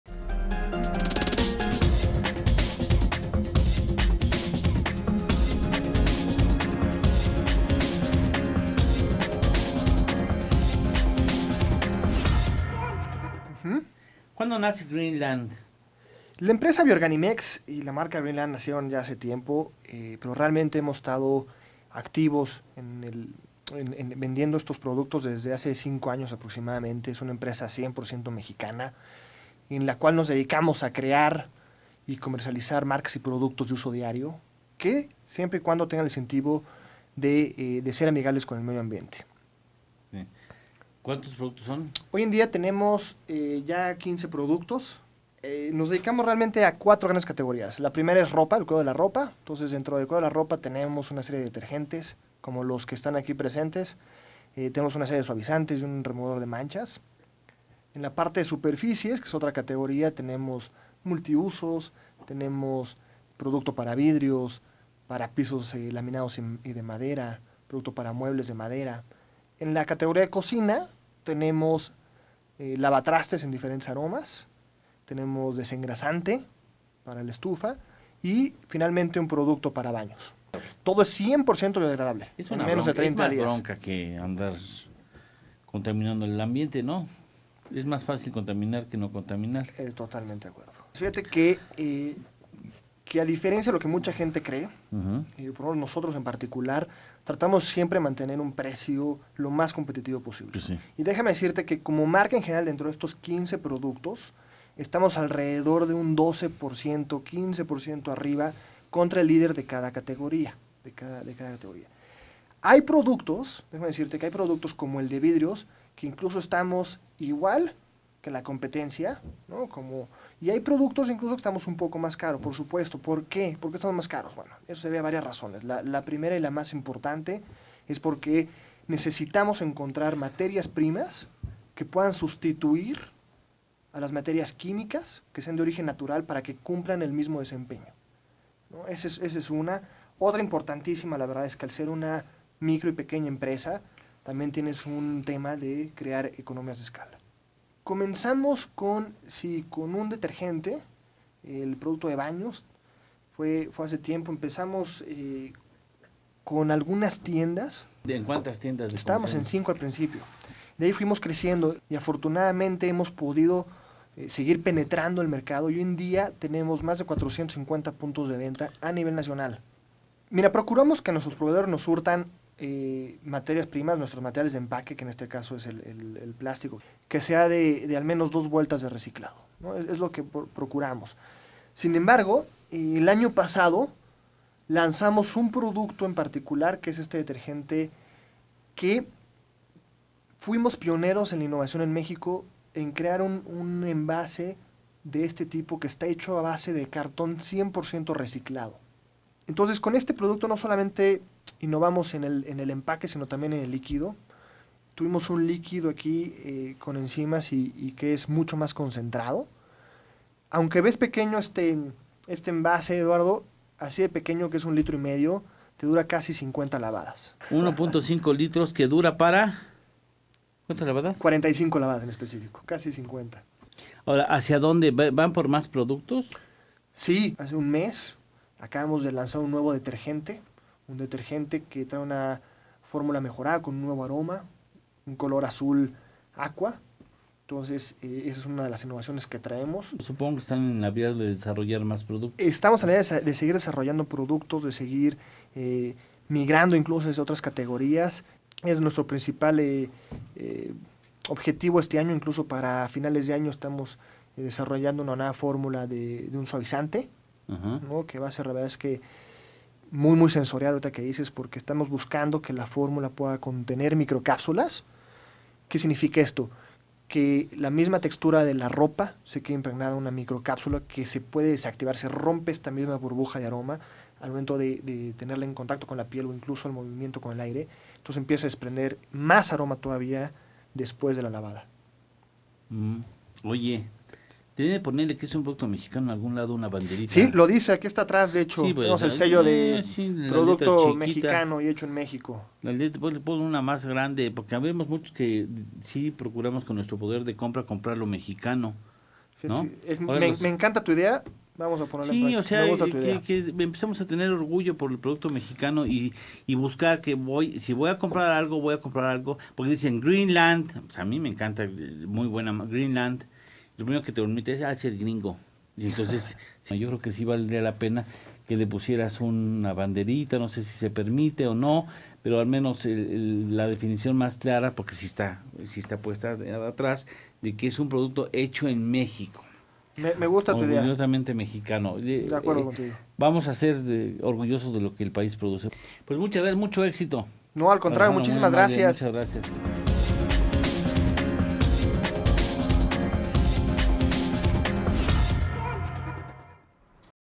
BIORGANIMEX
Eduardo Torreblanca nos explica acerca de Biorganimex, una empresa dedicada a crear marcas y productos de uso diario que no son nocivos para el medio ambiente.